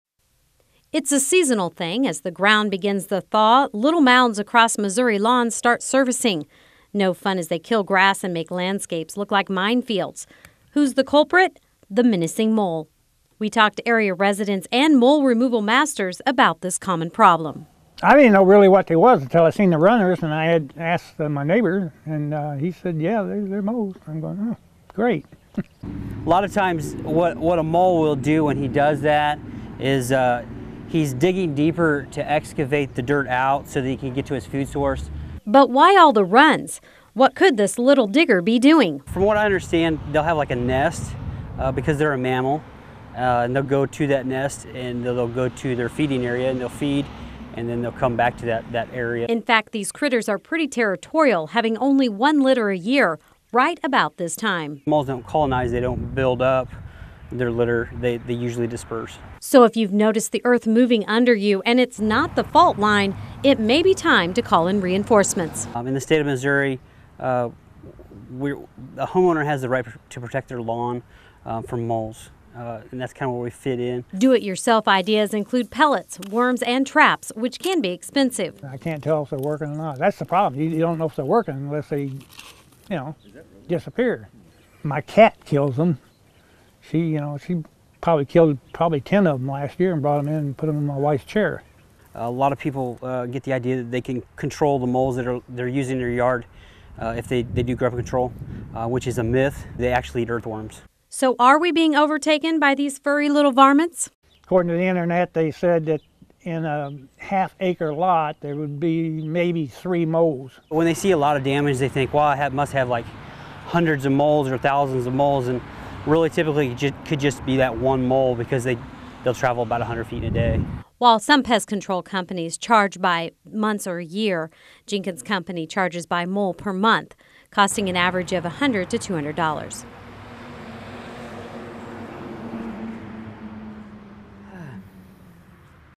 The Menacing Missouri Moles: how to combat the terrain troublemakers
Spring has sprung and with the thaw, comes those common mounds across the landscape. Moles are on the move. Julie Love speaks to residents and pest controllers in St. Joseph on how to manage the menacing mole.